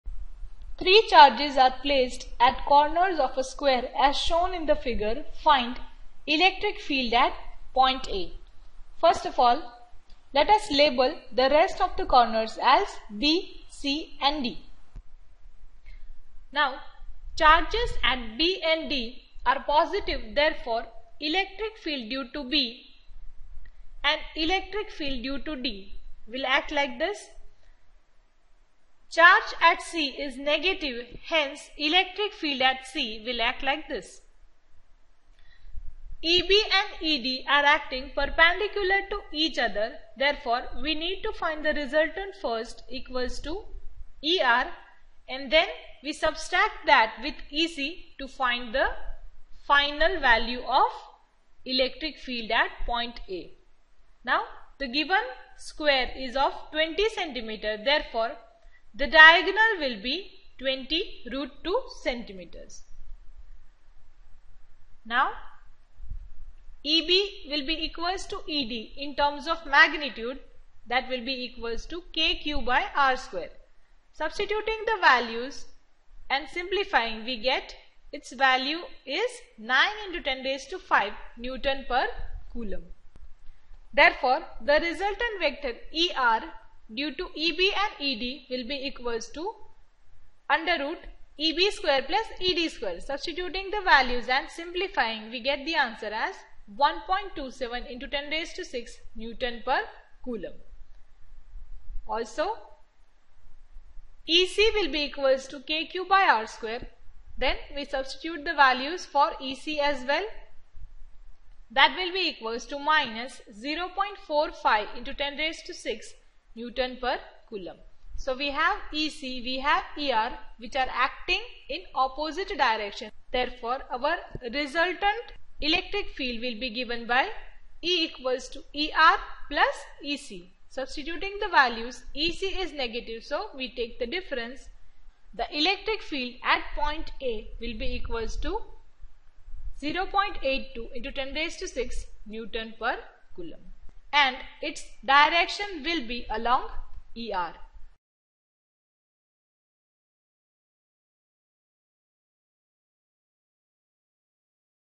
Resultant Electric Field in a given arrangement
Three charges are placed at corners of a square as shown in the figure. Find the electric field at point  A.